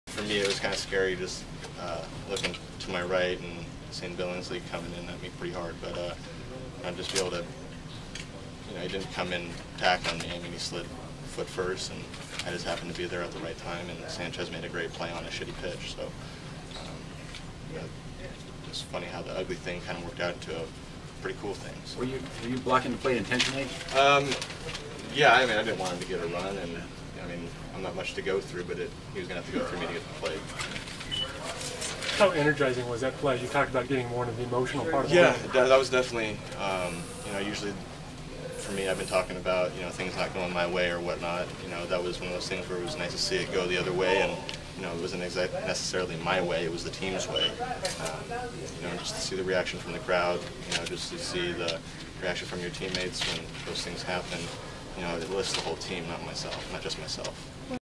Tim Lincecum talks about tagging Chad Billingsley out at the plate
San Francisco Giants pitcher Tim Lincecum talks about a key play against the Los Angeles Dodgers.